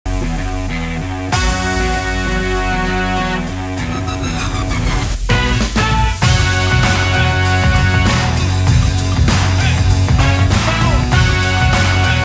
Vitesse INCROYABLE au Sevens !
Les Fidjiens nous montrent à quel point ils sont rapides dans ce clip incroyable du Wellington Sevens !

Pour des nouvelles, histoires, faits saillants, aller sur notre website officiel

Pour licencier des images de World Rugby, veuillez contacter IMG REPLAY ➡️